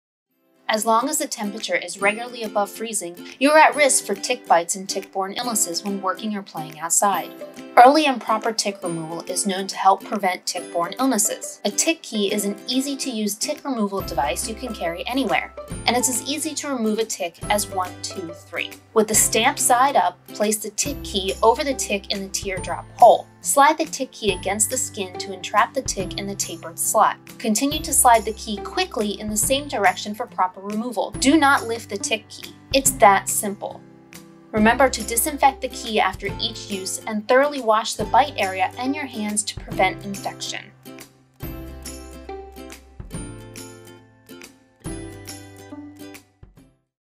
How to Use a Tick Key
A tick key can help easily remove ticks and help prevent Lyme disease.